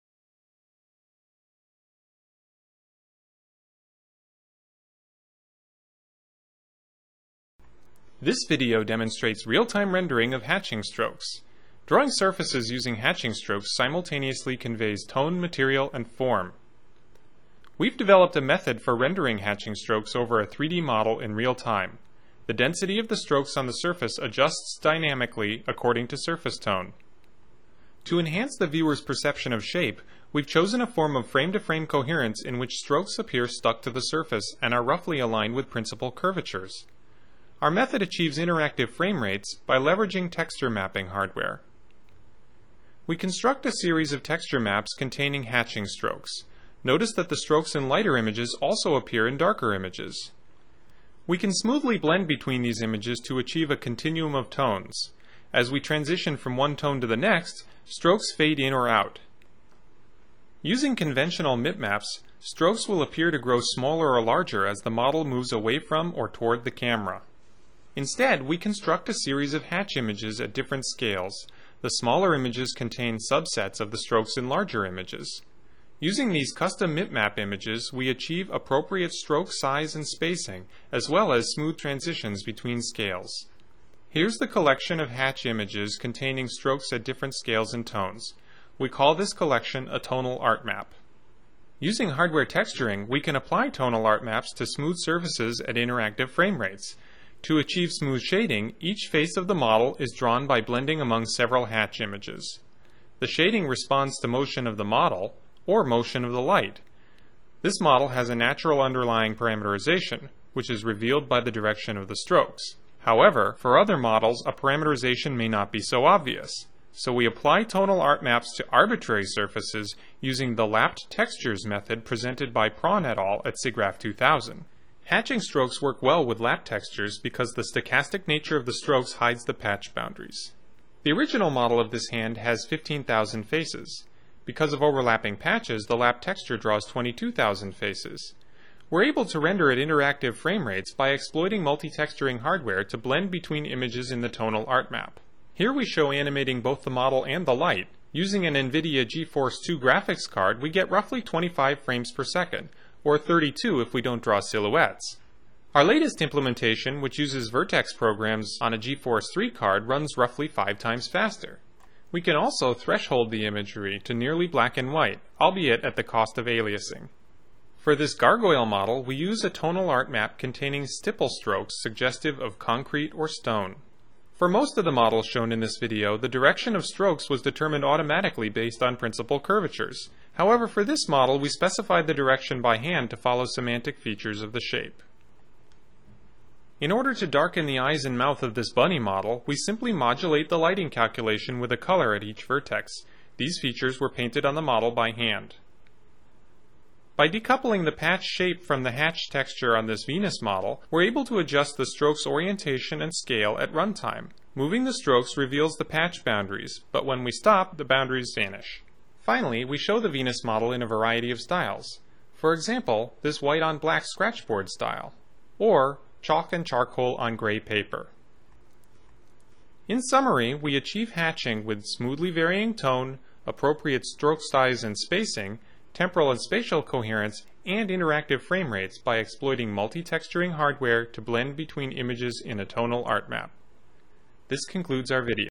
Real-time hatching (SIGGRAPH 2001)
Video accompanying the technical paper:
Emil Praun, Hugues Hoppe, Matthew Webb, Adam Finkelstein.
Real-time hatching.
ACM SIGGRAPH 2001 Proceedings, 581-586.

Abstract:

Drawing surfaces using hatching strokes simultaneously conveys material, tone, and form. We present a real-time system for non-photorealistic rendering of hatching strokes over arbitrary surfaces. During an automatic preprocess, we construct a sequence of mip-mapped hatch images corresponding to different tones, collectively called a tonal art map. Strokes within the hatch images are scaled to attain appropriate stroke size and density at all resolutions, and are organized to maintain coherence across scales and tones. At runtime, hardware multitexturing blends the hatch images over the rendered faces to locally vary tone while maintaining both spatial and temporal coherence. To render strokes over arbitrary surfaces, we build a lapped texture parametrization where the overlapping patches align to a curvature-based direction field. We demonstrate hatching strokes over complex surfaces in a variety of styles.